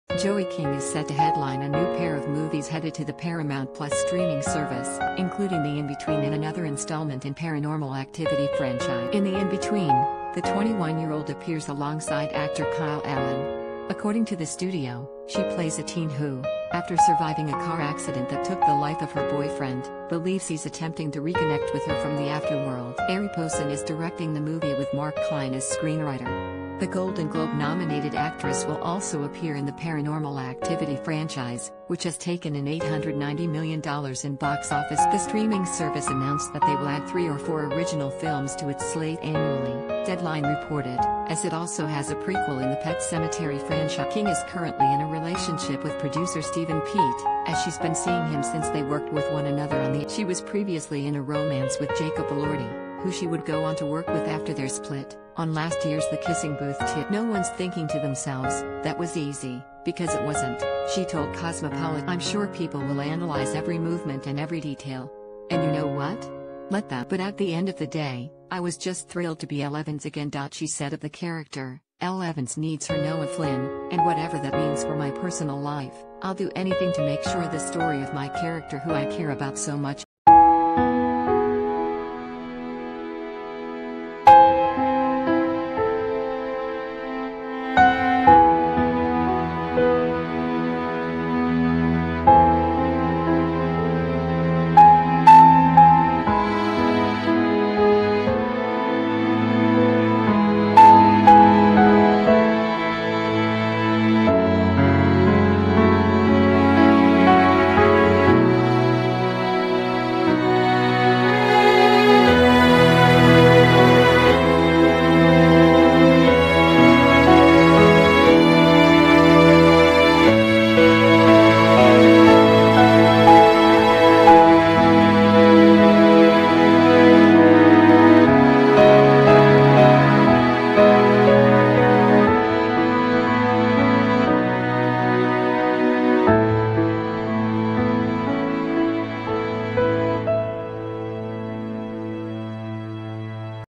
Joey King headlines new pair of films headed to Paramount+
Joey King is set to headline a new pair of movies headed to the Paramount+ streaming service, including The In Between and another installment in Paranormal Activity franchise.In The In Between, the 21-year-old appears alongside actor Kyle Allen. According to the studio, she plays a teen who, 'after surviving a car accident that took the life of her boyfriend, believes he’s attempting to reconnect with her from the after world.'Arie Posin is directing the movie with Marc Klein as screenwriter.    The Golden Globe-nominated actress will also appear in the Paranormal Activity franchise, which has taken in $890 million in box office totals.The streaming service announced that they will add three or four original films to its slate annually, Deadline reported, as it also has a prequel in the Pet Sematary franchise on the way.  King is currently in a relationship with producer Steven Piet, as she's been seeing him since they worked with one another on The Act.She was previously in a romance with Jacob Elordi, who she would go on to work with after their split, on last year's The Kissing Booth 2.    'No one’s thinking to themselves, "That was easy," because it wasn’t,' she told Cosmopolitan last year. 'I’m sure people will analyze every movement and every detail. 'And you know what? Let them. But at the end of the day, I was just thrilled to be Elle Evans again.'She said of the character, 'Elle Evans needs her Noah Flynn, and whatever that means for my personal life, I’ll do anything to make sure the story of my character who I care about so much is complete.'